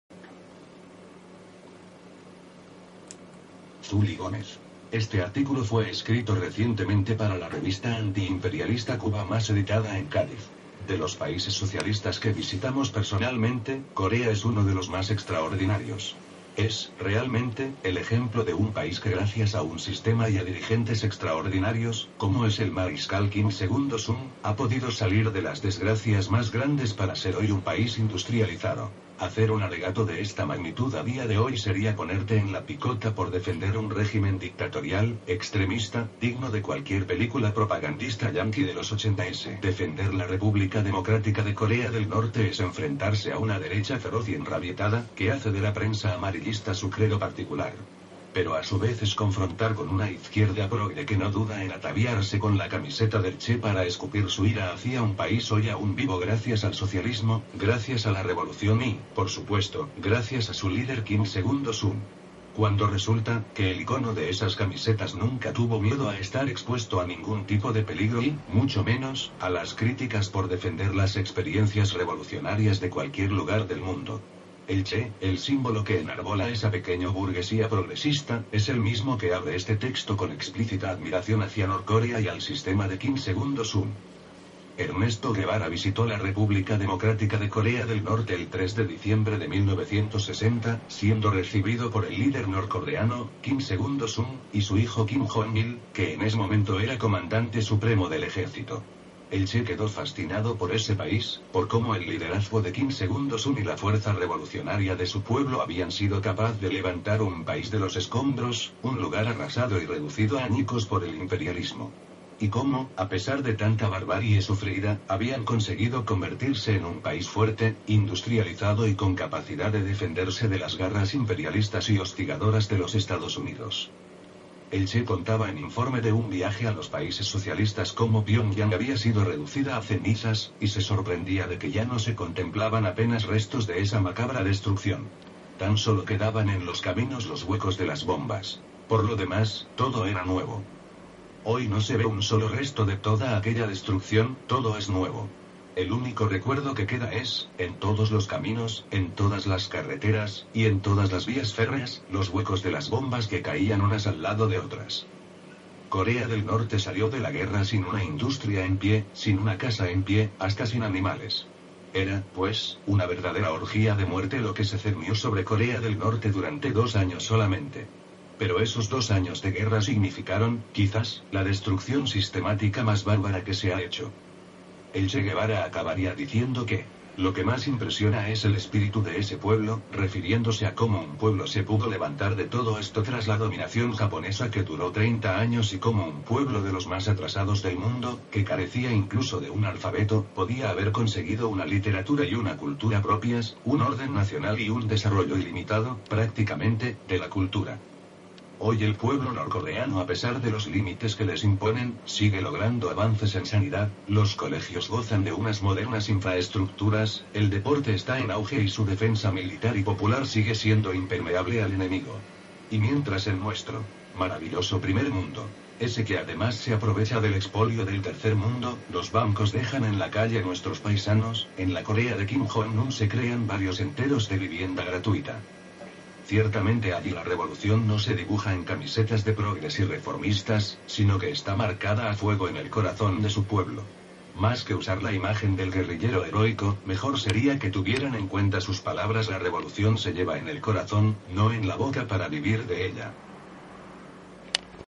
EL CHE Y COREA DEL NORTE
RECORDANDO LA  VISITA  DEL CHÉ  GUEVARA A COREA DEL NORTE.